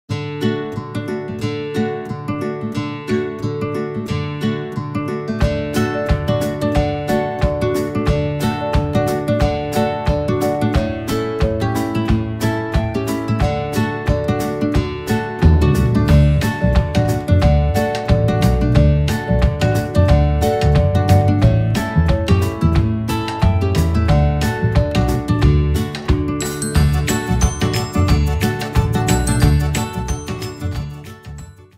Finalmente LA SIGLA DEL MIO CANALE ♡
Ecco la sigla del mio canale creata de DENISE DE SPIRITO. Cosa ne pensate ?  Fatemi sapere lasciando un bel commentino !io la amo !!!♡*.*